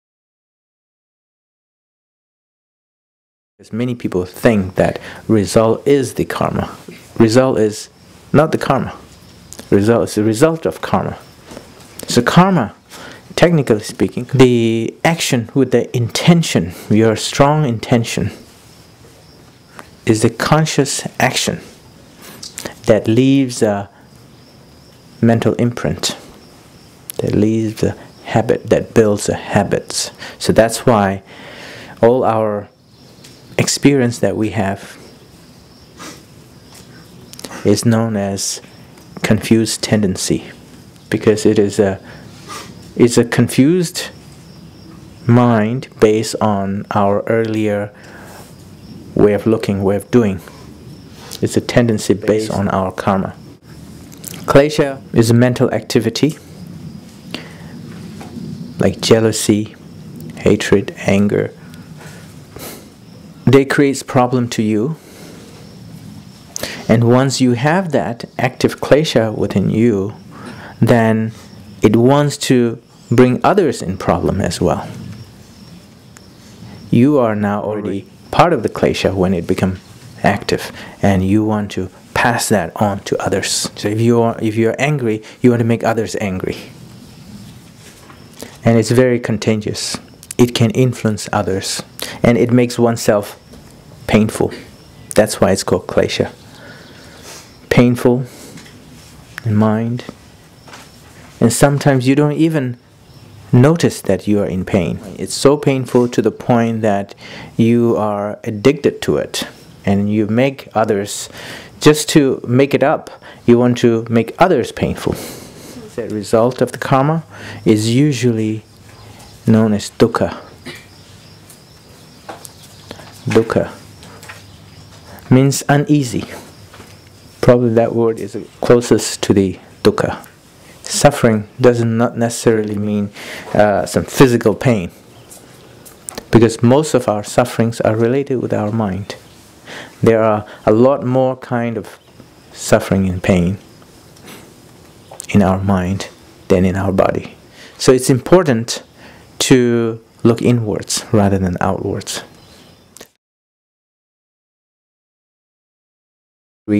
Karma, Kleshas, and Dukha
Trungram Gyaltrul Rinpoche explains the difference between karma, kleshas and duhka: Karma is a conscious action that forms mental habits; kleshas are mental activities, such as anger or jealousy; dukha is the result of karma.